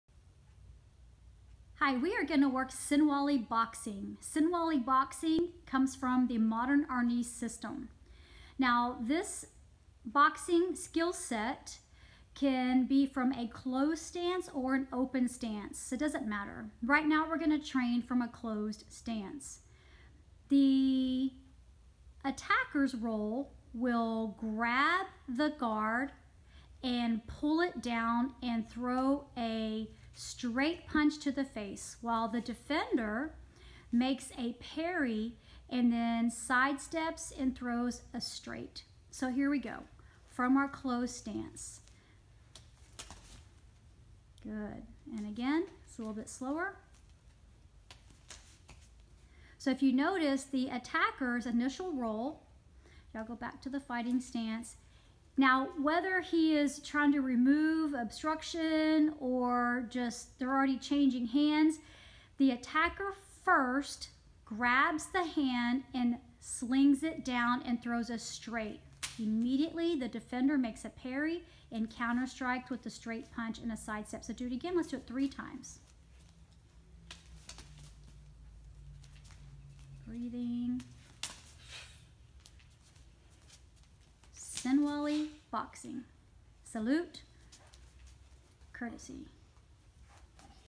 Sinwali Boxing
From a boxers stance your opponent will grab your guard and throw a straight punch to your chin, the moment that punch comes your way you will parry it and side step and throw a immediate counter strike, a straight punch to their jaw (or rib).
This is Sinwali Boxing. It is simply executed when the fight has already engaged and you are in punch range and your opponent grabs your guard...

Watch this again and again. Pay attention to the body movements, the party blocks, the timing.
If you don’t have a partner then shadow box and simulate the moves by a would be attacker.

Train this for 15-20 minutes. Be nice to your partner and don’t parry to hard as being slapped for 15-20 minutes might get old... instead, train for 3 minutes on one side then switch to a open stance or a closed stance for another 3 minutes. Then switch hands for 3 minutes and repeat on other side. Then it’s your turn to be the attacker.

Be sure to breathe out and keep those guards up. Find something positive to say to your training partner and if you don’t have one. Then feel good about yourself putting that work in. Give yourself time to develop these skills.

Watch these videos on your TV on a big screen and take notes. This is your time to develop much!:)

P.S. my wonderful students demonstrating here had a full evening of filming Krav Maga, Keysi, Jiujitsu, Karate and Arnis. By the time our videos came to an end and we bowed, We almost didn’t remember what system we were working in and often caught us off guard with the bow etiquette as each system has its bow, LOL! You can see this here in this video, I laugh and have so much joy in my heart when I see it. Gotta love the etiquette in the Arts!:)))